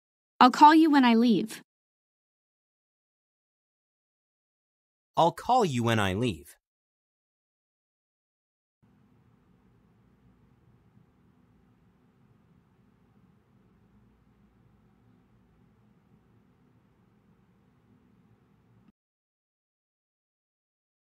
How to say "I'll call you when I leave."
Male Voice:
I'll call you when I leave.